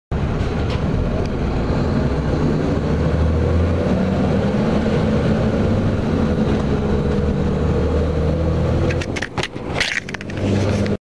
EPIC FAIL While Filming Bus
A ride video turned into a failure of such EPIC proportions that you have to see it to believe it.  Trust me, you will be laughing so hard when you see it.  Just imagine how much worse it have turned out if I didn't use the camera strap like it was designed for.